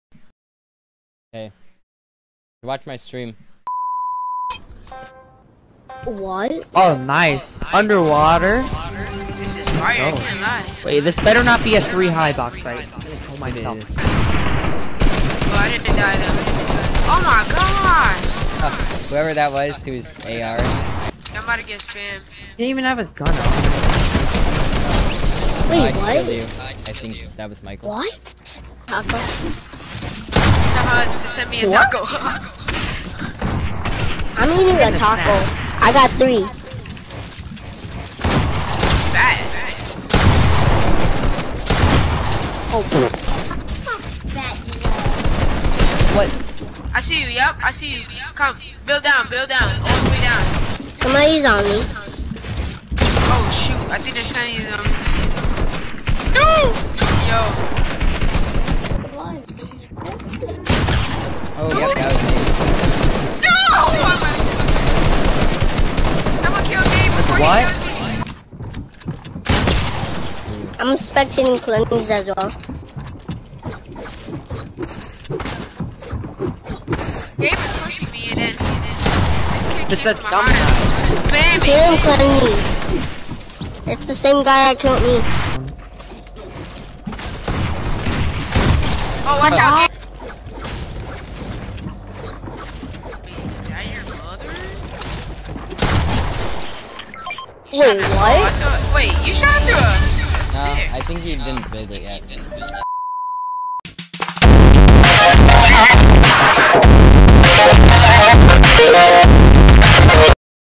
Fighting in Boxes.
Box Fights.
This is a short video of me and my friends playing Boxfights in Creative Mode.

I try to stream when I can. Maybe you will watch.

Thanks.